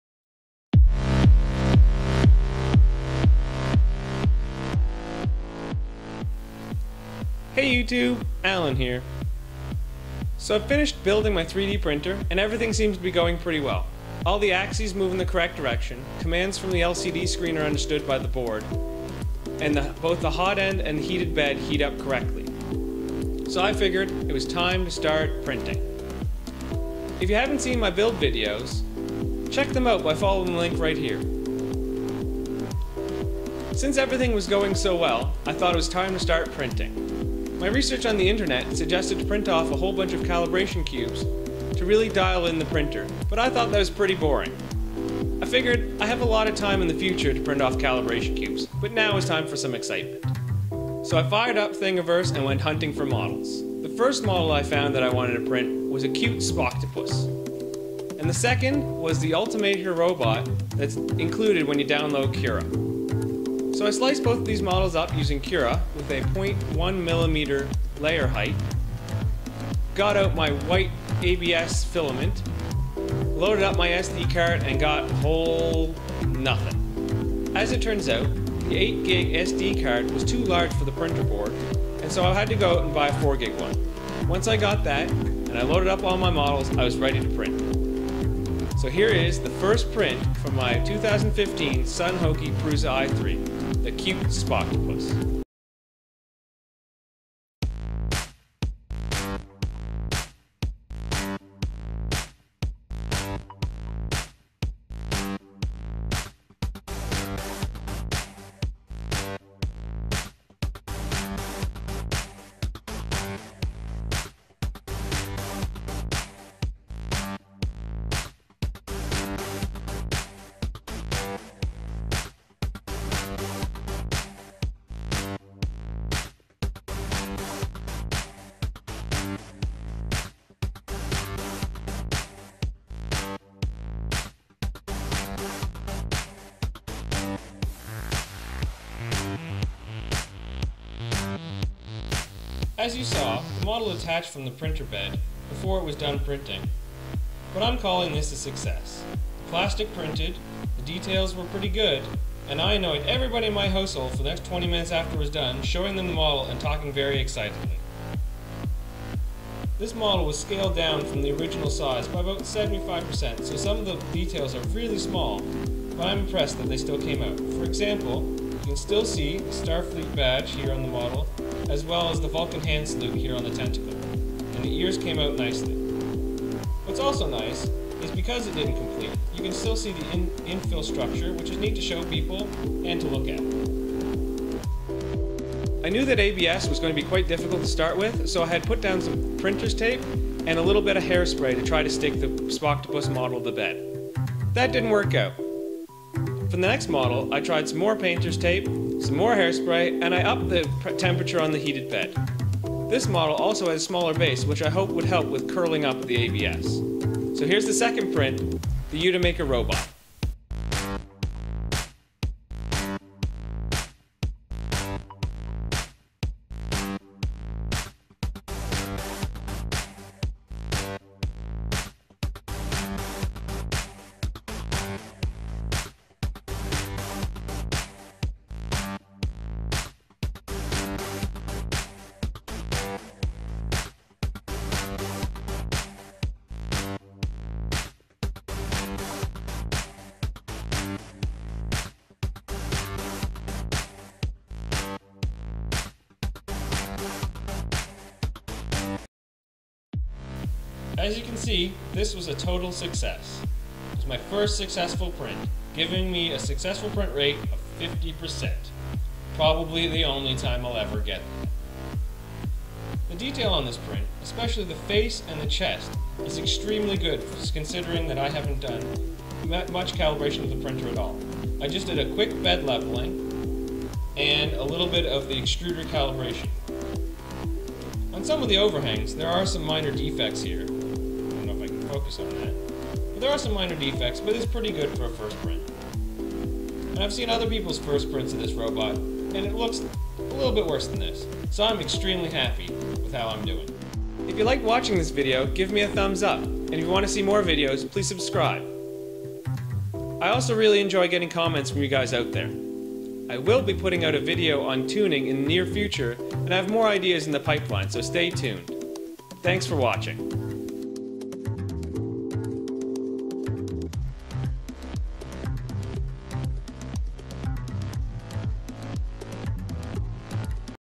Sunhokey Prusa i3 3D Printer | First Prints
Hey Youtube,

I am back with a video featuring the first prints from my Sunhokey Prusa i3 3D Printer (2015 edition). Check out the results of the first plastic ever pushed through the hot end!!